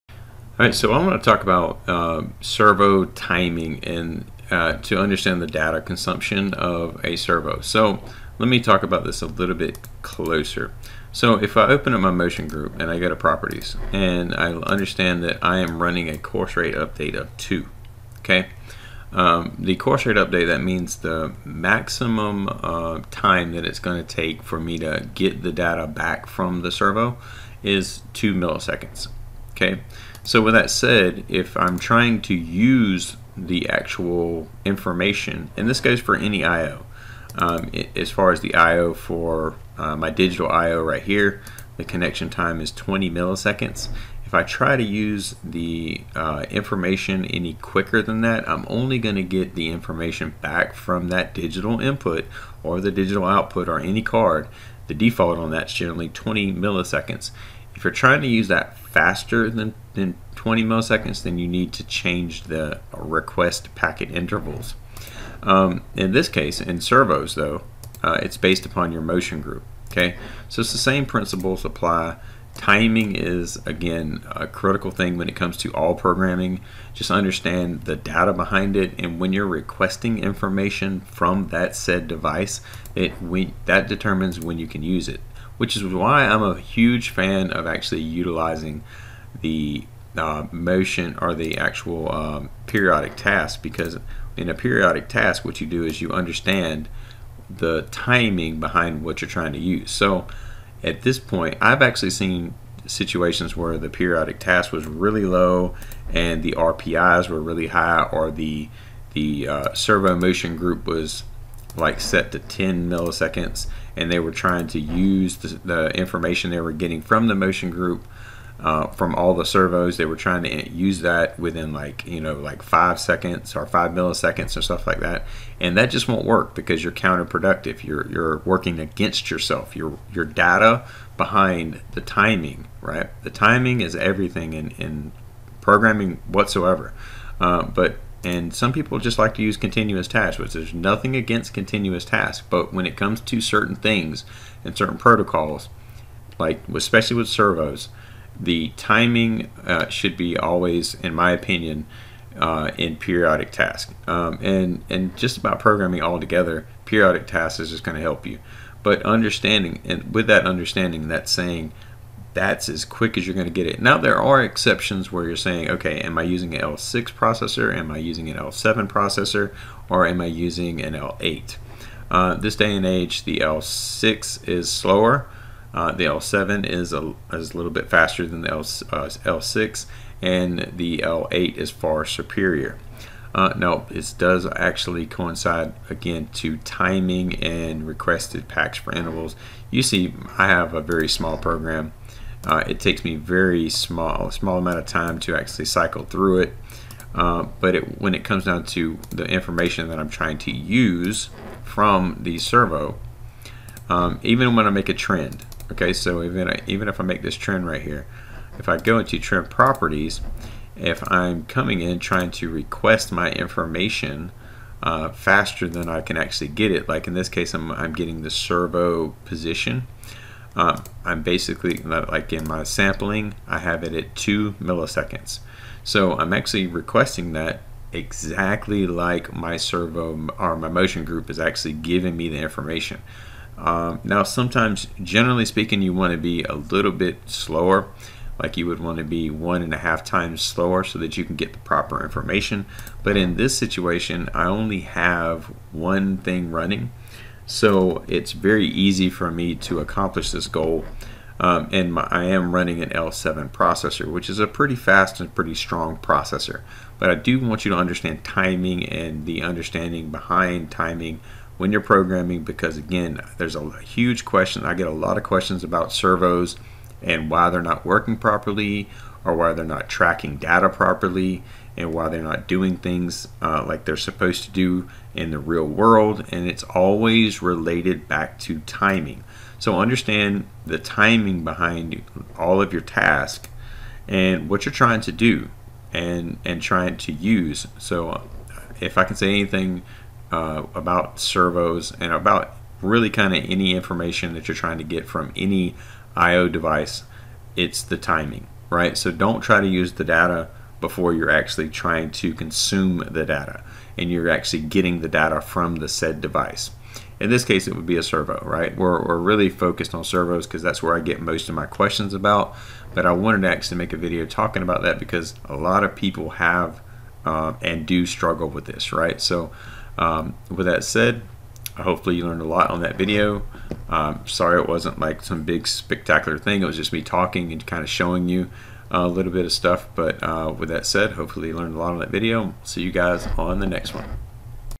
Rockwell Automation's Studio 5000 Servo Controls - Understanding Timing & Data use
Studio 5000 Servo Controls - Understanding Timing & Data use

This video was made to discuss timing and how it relates to writing plc logic or understanding why a servo might be experiencing weird issues that do not make sense, it is very possible that timing is one of the causes.

Rockwell Automation's Studio 5000 Servo Controls are a key component of many industrial automation systems, providing precise control over the movement of servo motors in real-time. In this article, we will explore the concepts of timing and data use in Studio 5000 Servo Controls and how they play a critical role in the operation of these systems.

To understand timing and data use in Studio 5000 Servo Controls, it is first important to understand the basic operation of a servo motor. A servo motor is a type of rotary actuator that uses feedback to control the position, velocity, or torque of the motor. In a servo system, the servo motor is connected to a control system, which sends commands to the motor in the form of electrical pulses. These pulses control the position of the motor by determining the duration of the pulse, or pulse width.

In Studio 5000 Servo Controls, timing and data use play a critical role in the operation of the servo motor. The control system uses data from various sensors, such as encoders and position feedback devices, to determine the current position and velocity of the servo motor.

This data is used to calculate the required pulse width for the servo motor to achieve the desired position or velocity.

The timing of the pulse width is also important in Studio 5000 Servo Controls. The control system sends pulses to the servo motor at a specific frequency, known as the control frequency. The control frequency determines the speed at which the servo motor can respond to commands and make adjustments to its position.

A higher control frequency allows the servo motor to respond more quickly to changes in the system, while a lower control frequency results in slower response times.

In addition to the control frequency, the timing of the pulse width is also affected by the servo motor's response time. The response time is the time it takes for the servo motor to respond to a change in the pulse width.
A longer response time means that the servo motor will take longer to make adjustments to its position, while a shorter response time allows for faster adjustments.

The use of data in Studio 5000 Servo Controls is also critical for ensuring the accuracy and precision of the servo motor's movement. The control system uses data from various sensors to calculate the required pulse width for the servo motor to achieve the desired position or velocity.

This data is also used to adjust the control frequency and response time of the servo motor as needed to ensure that the servo system is operating at optimal performance.

In conclusion, timing and data use are key concepts in the operation of Rockwell Automation's Studio 5000 Servo Controls. The control frequency, pulse width, and response time of the servo motor all play a critical role in the accuracy and precision of the servo system, while data from various sensors is used to ensure optimal performance.

Understanding these concepts is essential for the effective operation and maintenance of servo systems in industrial automation applications.

This is mainly a discussion that shows important points about using data within the program from the course rate update time or the RPI from a device.